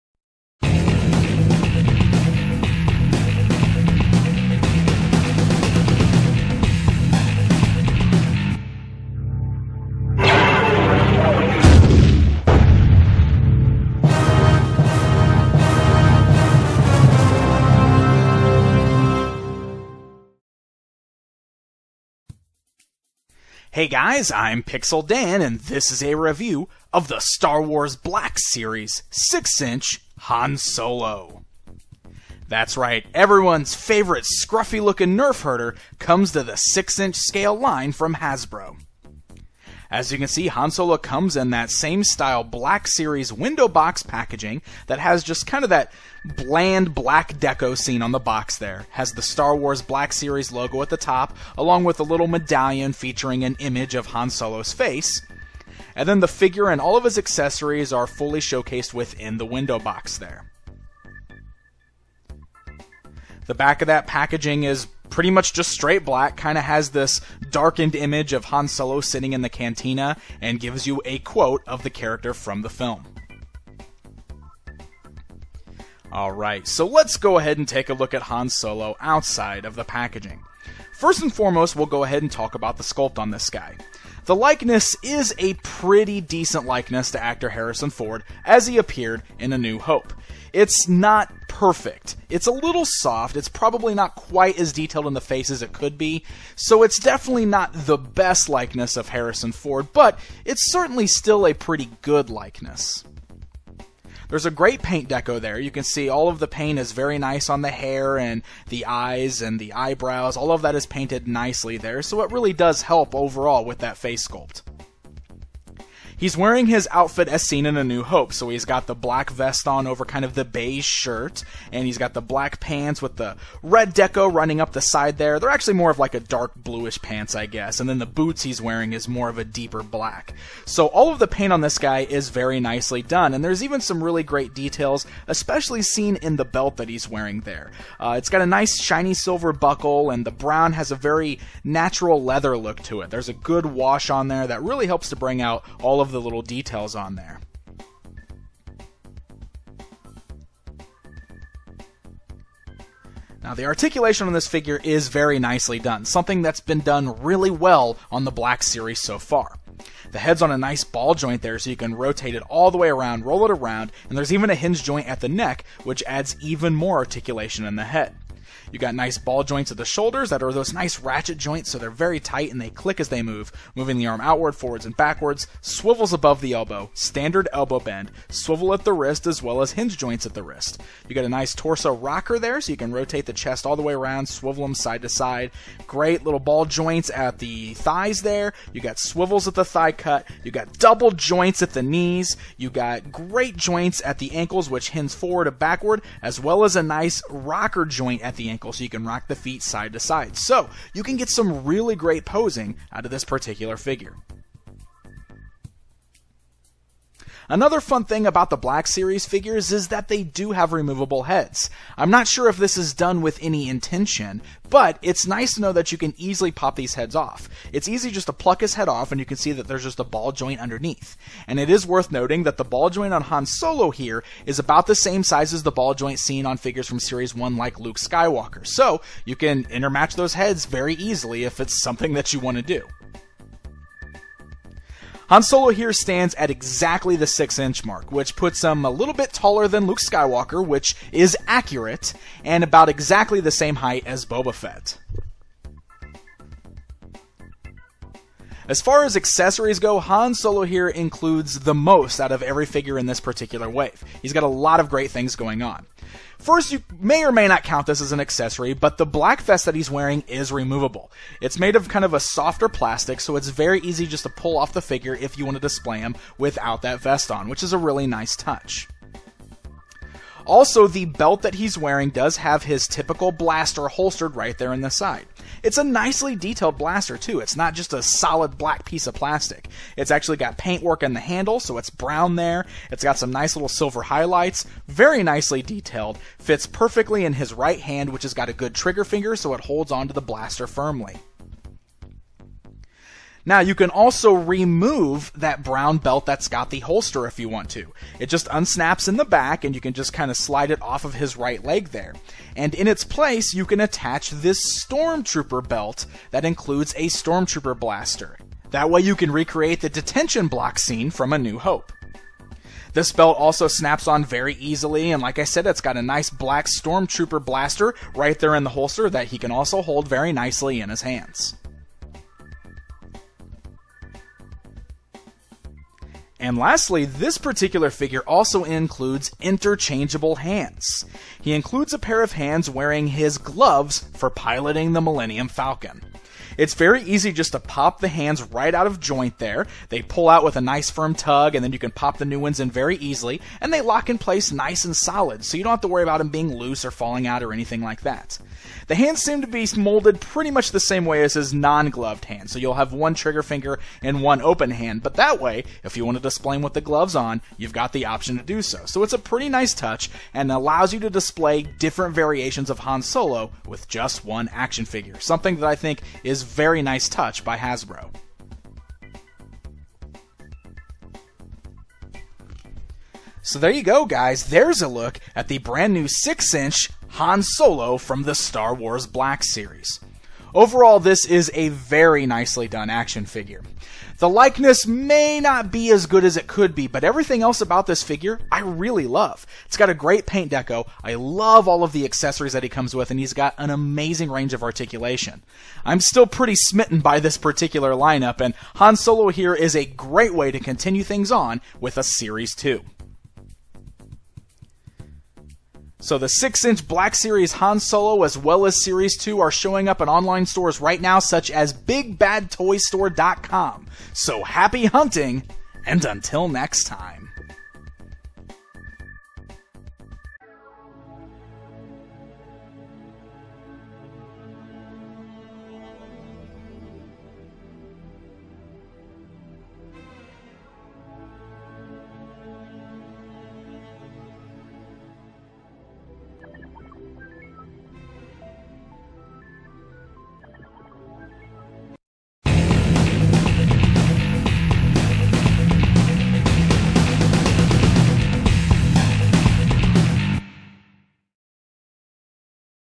Hasbro Star Wars Black Series 6" Han Solo Figure Video Review
Star Wars Black Series Wave 2 is available now at Big Bad Toy Store

The Black Series Wave 2 is here! Join Pixel Dan for a look at Han Solo as seen in A New Hope!